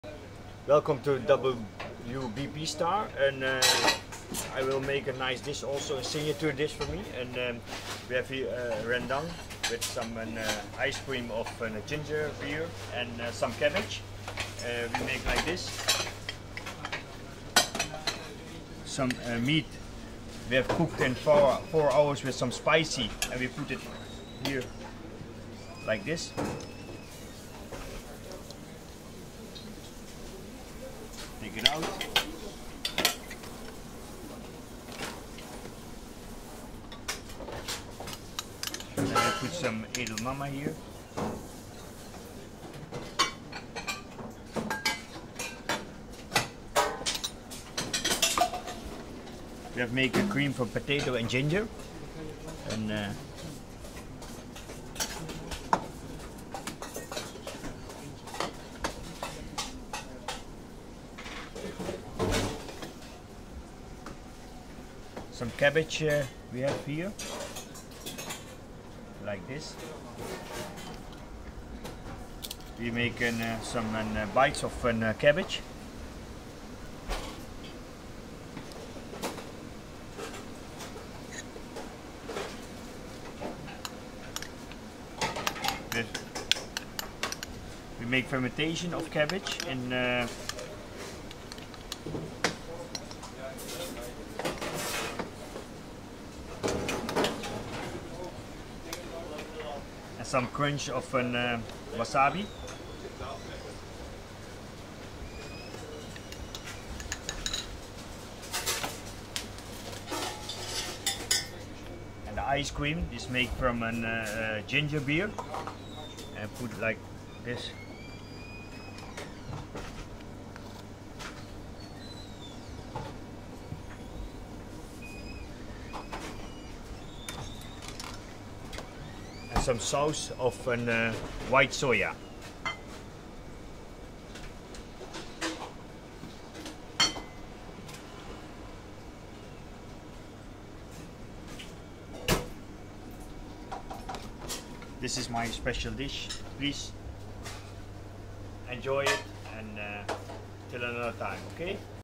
Soenil Bahadoer prepares a signature dish at his Michelin star restaurant
Chef Patron Soenil Bahadoer prepares a signature dish at restaurant De Lindehof in Nuenen, The Netherlands.